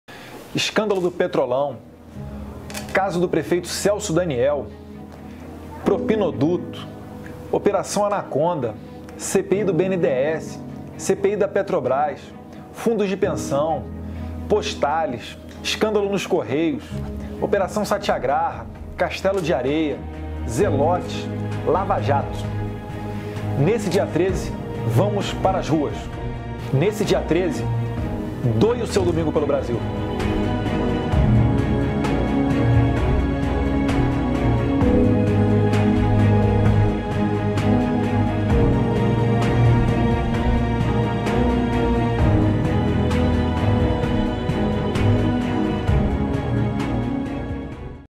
O POVO NAS RUAS!
DIA 13 DE MARÇO
O povo brasileiro precisa reconhecer sua força.
As mudanças só acontecerão com a efetiva participação do povo nas ruas!